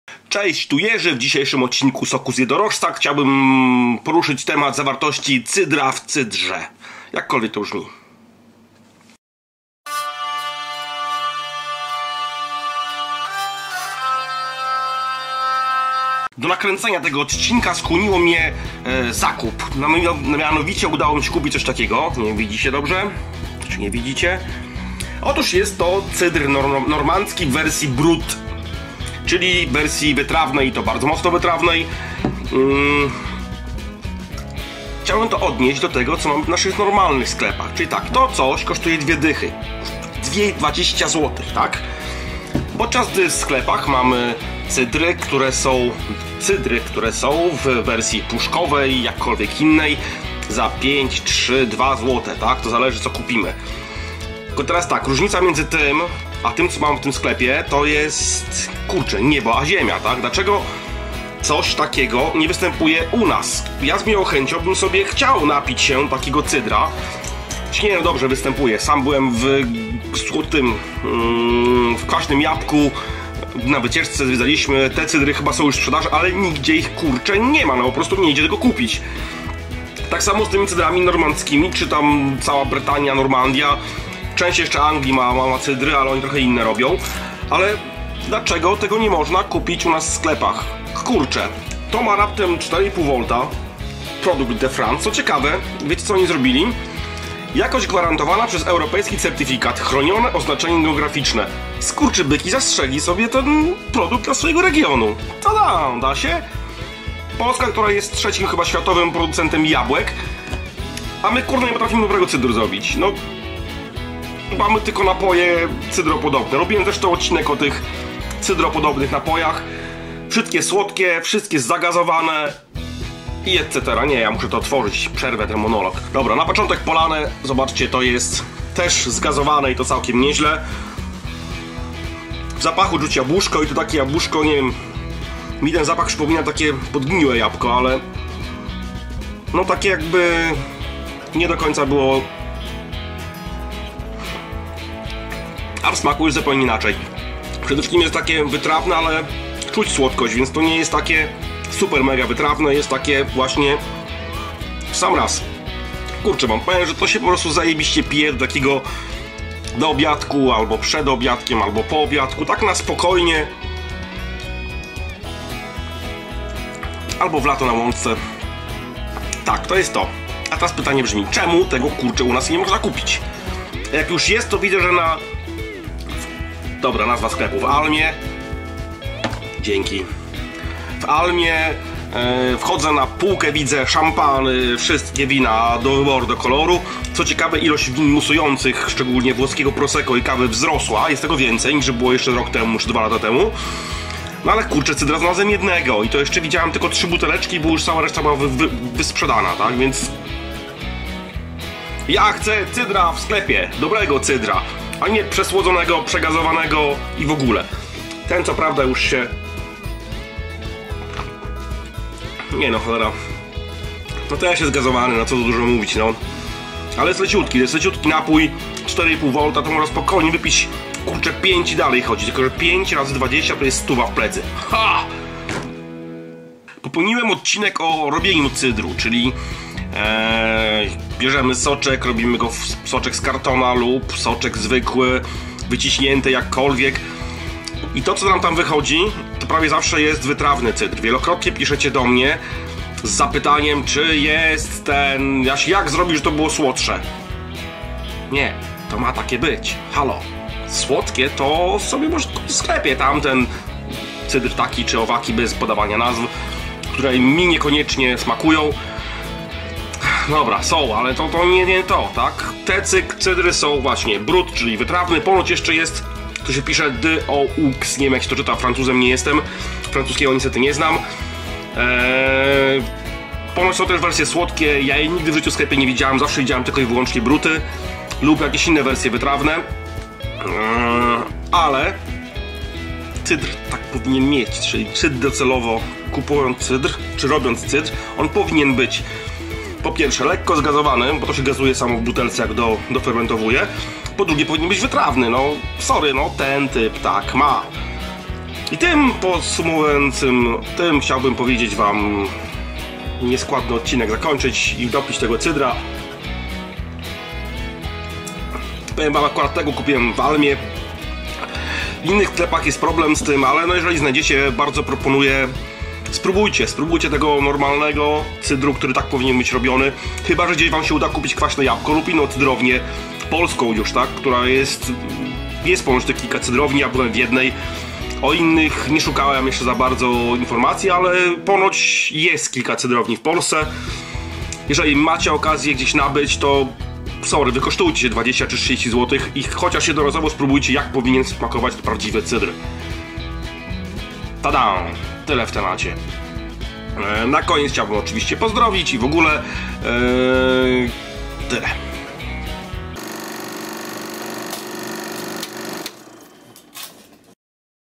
Zawartość cydru w cydrze
smutny monolog nad butelką normandzkiego cydru...